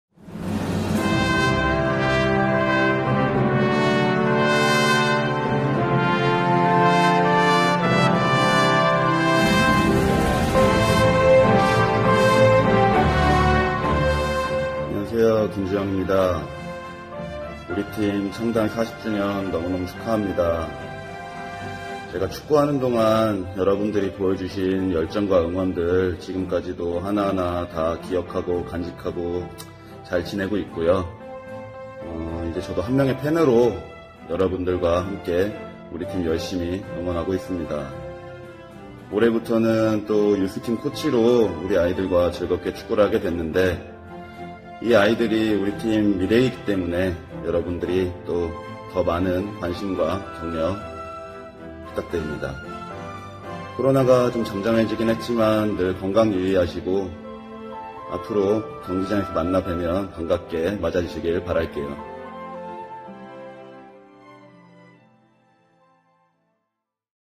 김주영의 FC서울 창단 40주년 축하 영상
FC서울에서 선수로 함께했고
현재 FC서울 U-15팀 코칭스태프인 김주영 코치가
구단 창단 40주년 축하 메시지를 전해왔습니다.